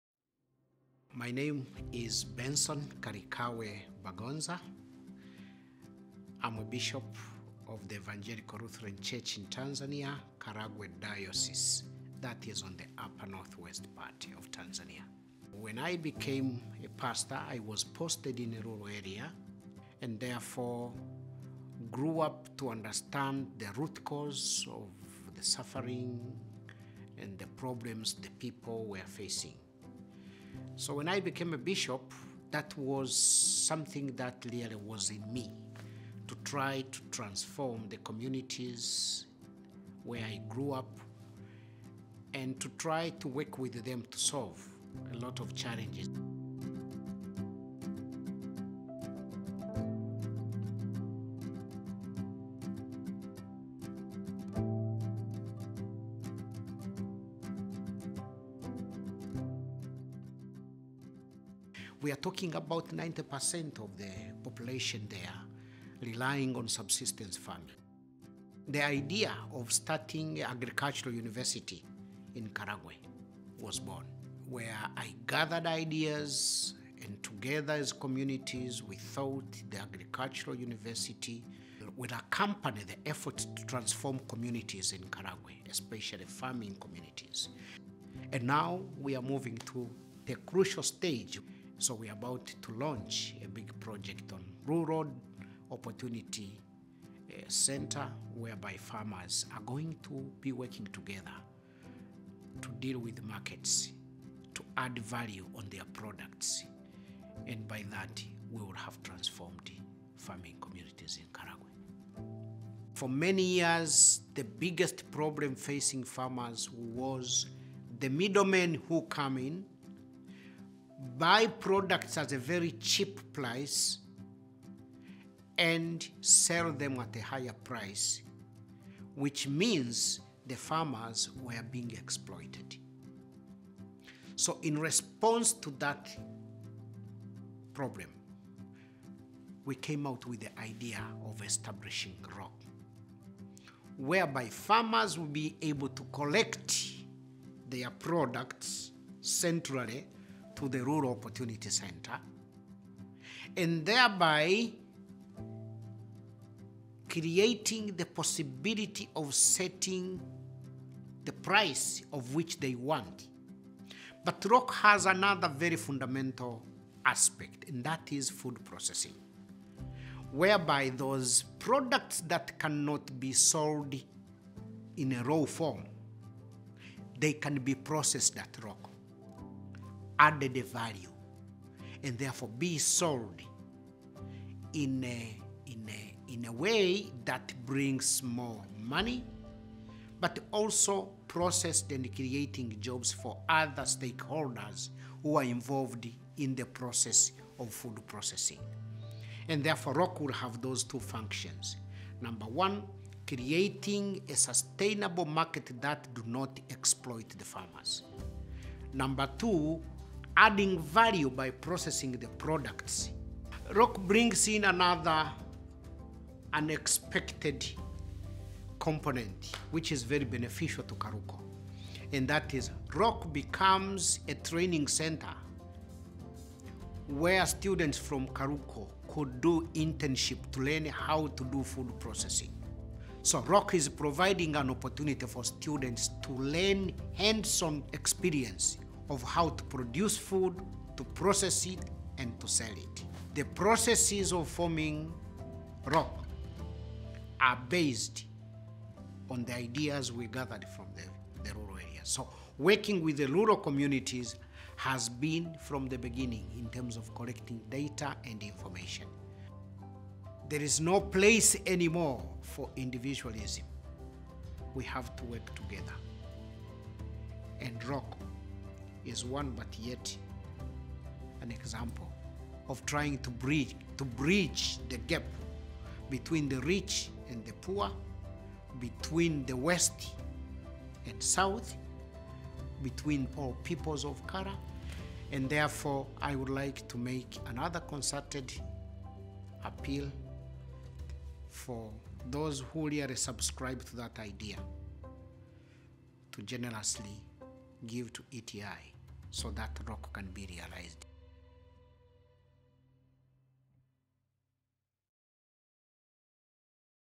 Help Build Rural Opportunity Centers Now!
Dr. Bagonza shares his vision and plans for economic and social development in Karagwe. Tune in and be part of the story.